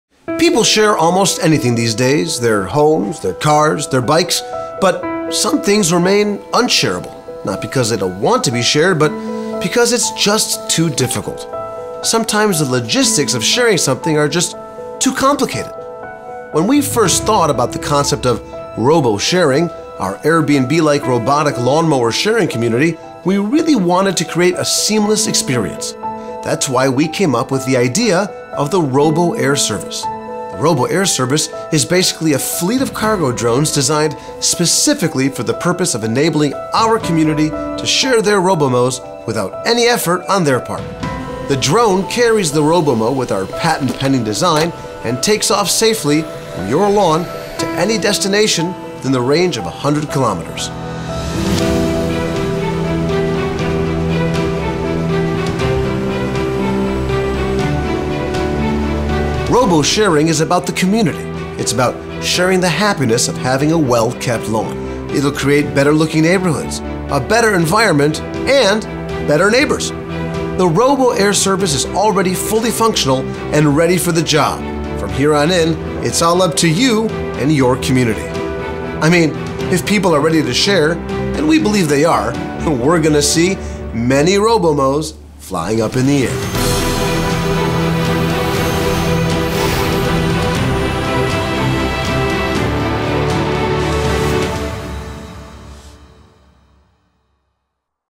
Robomow presents: RoboAir, the first ever shared Robotic Mower
Check out Robomow's new air service, the RoboAir, this advanced robot drone will take Robomow from your garden to the next garden in the neighborhood so the entire street can share one mower.